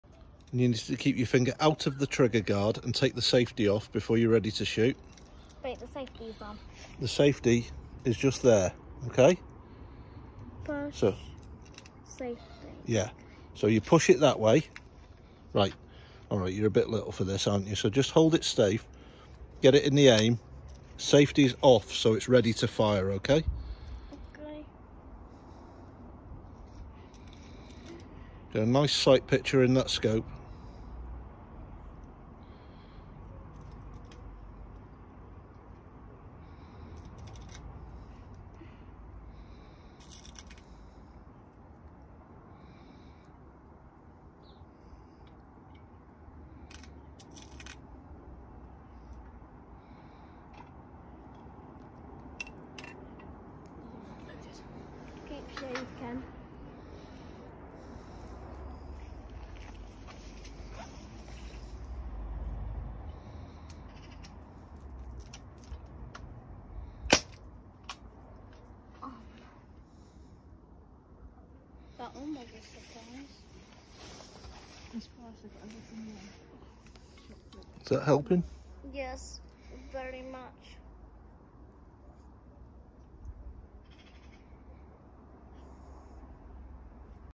Konr trying to hold the rifle
Even the tiny Crossman 1077 can be a little on the big side when you are seven years old.
Rest options considered but he wanted to try and do it himself
6ftlbs plinker
Crossman 1077 in .177